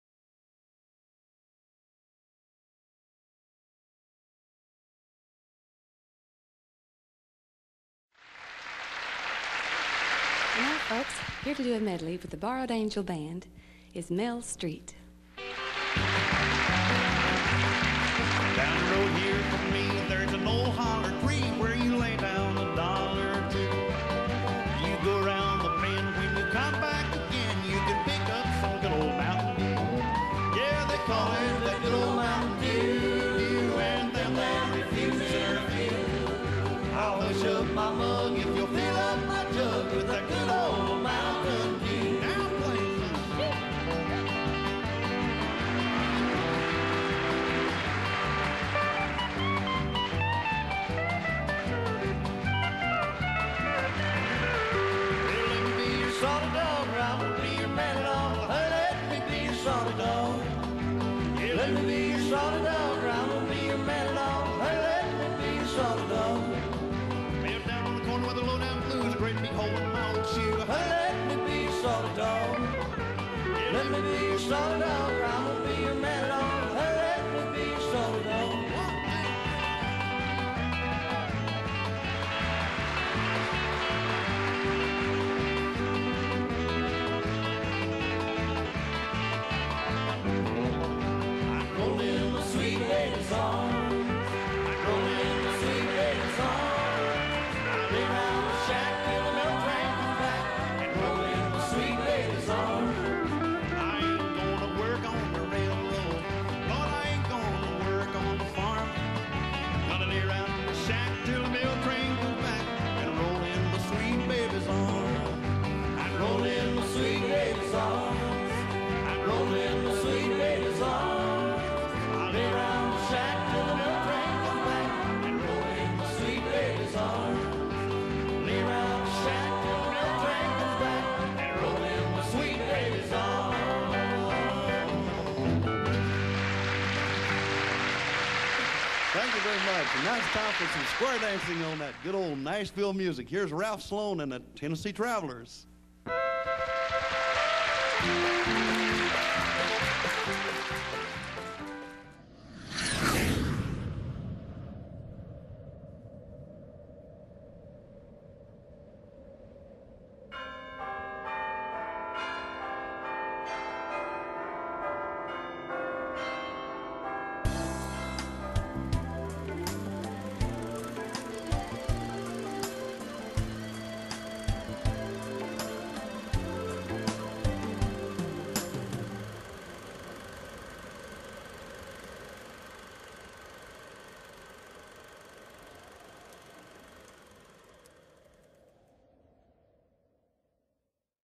MEL STREET LIVE VIDEO - Bluegrass Medley - 1976
A classic Mel Street appearance.  One of few television appearances that Mel ever did.  Here he is with his band which includes the now well known Nashville session musicians, Mike Johnson on Steel and Lester Earl Singer on Guitar and vocals.  They are regulars on the Country Family Reunion shows on RFD-TV and RURAL TV seen in the UK